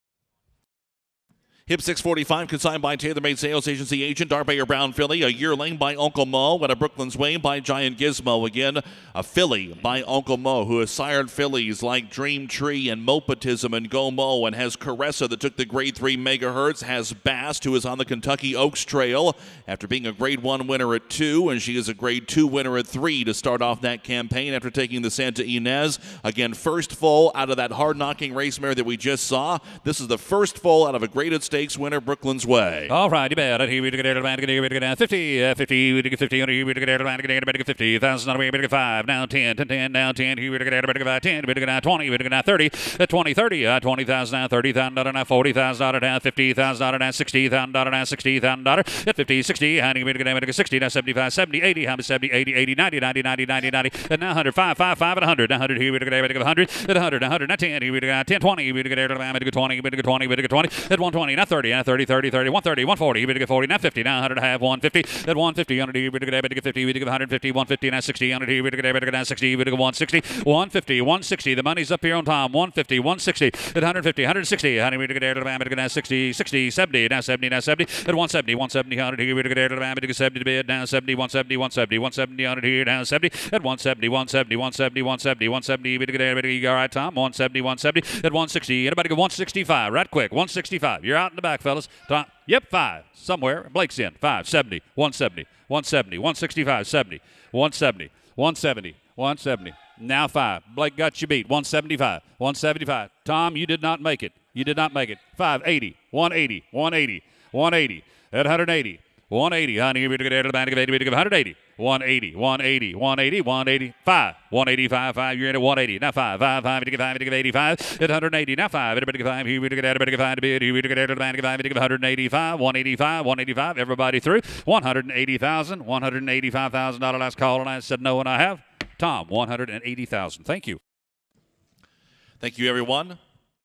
Clement L. Hirsch S. winner ADARE MANOR at Kentucky Winter Mixed
Grade 1 Winner ADARE MANOR is a Fasig-Tipton sales graduate!

NOTABLE WINS
G1 Clement L. Hirsch S. (2023)
G2 Santa Margarita S. (2023)
G2 Santa Maria S. (2023)
G3 Las Virgenes S. (2023)

CONNECTIONS
Owner: Michael Lund Petersen
Trainer: Bob Baffert
Breeder: Town & Country Farms and Gary Broad

SALE HISTORY

Sale: Kentucky Winter Mixed (2020)
Price: $180,000
Buyer: Walmac Farms / Gary Broad
Consignor: Taylor Made Sales Agency, agent

Sale: Selected Yearlings Showcase (2020)
Consignor: Gainesway, agent

Updated: 8/5/2023